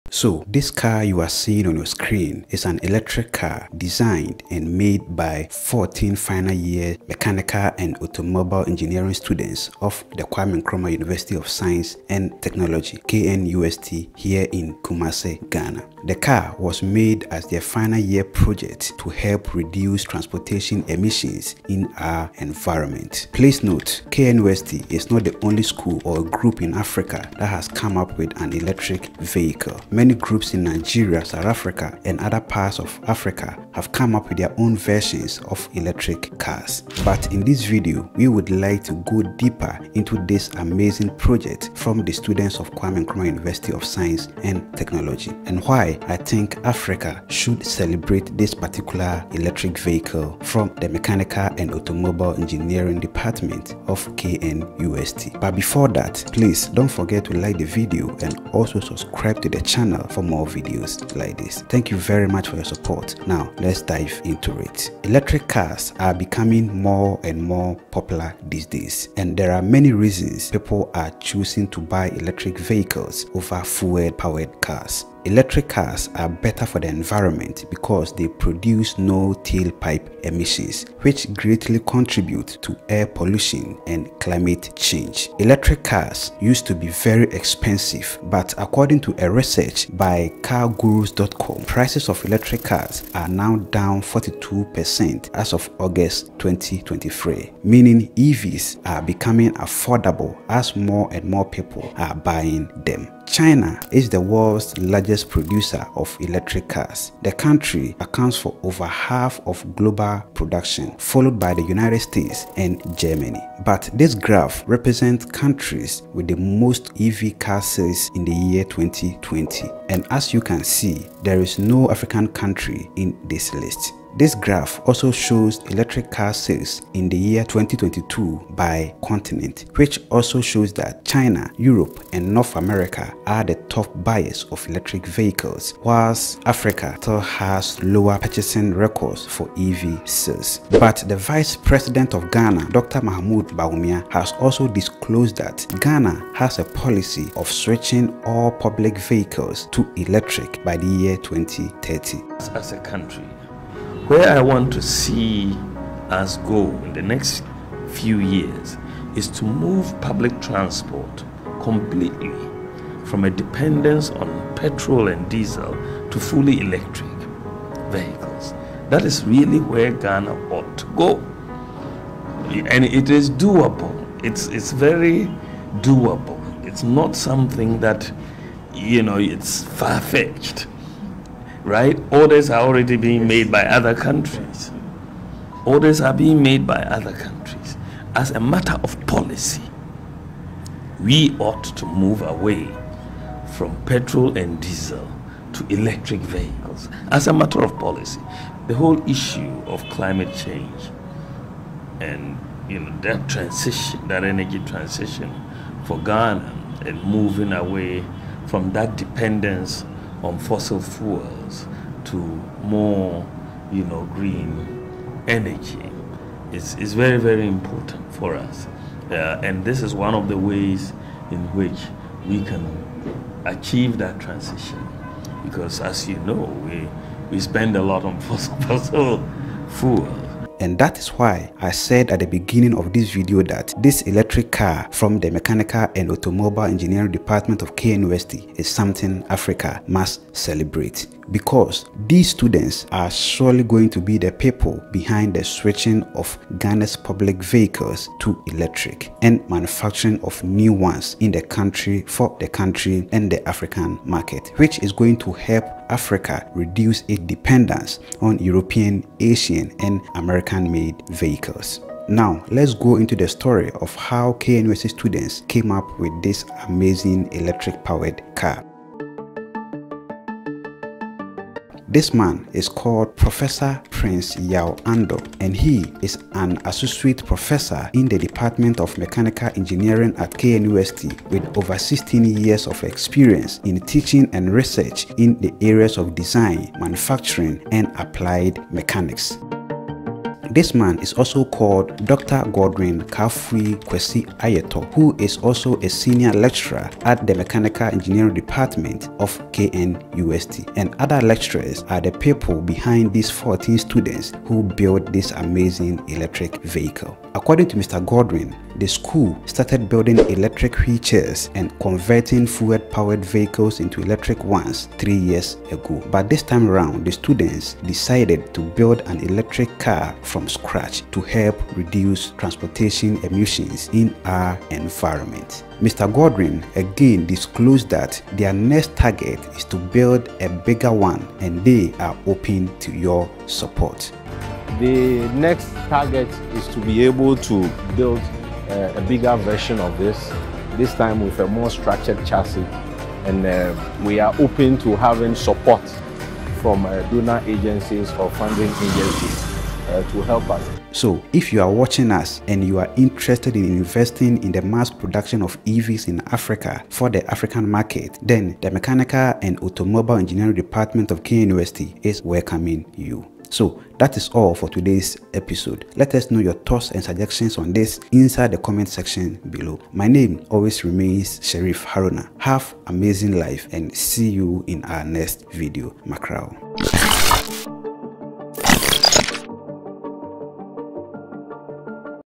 Ghanaian University Students Have Built a Fuel-Free Vehicle to help reduce emissions in Ghana
A group of fourteen Kwame Nkrumah University of Science and Technology (KNUST) final-year students have designed and built a MAC 4 electric vehicle they made the car as their final year project to help reduce transportation emissions in our environment

If you are or represent the copyright owner of materials used in this video and have a problem with the use of said material, please send me an email, , and we can sort it out.